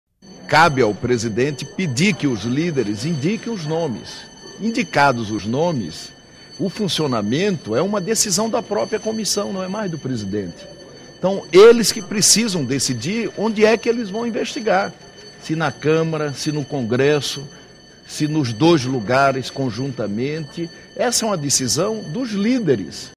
Renan: líderes decidirão qual CPI irá funcionar
O presidente do Senado, Renan Calheiros, confirmou para a próxima terça (6) reunião com os líderes partidários do Congresso para que indiquem os nomes de senadores e deputados que irão compor a CPI Mista da Petrobras. Renan disse que caberá, então, aos líderes decidirem se as investigações serão realizadas por uma CPI Mista ou por uma CPI formada apenas por senadores.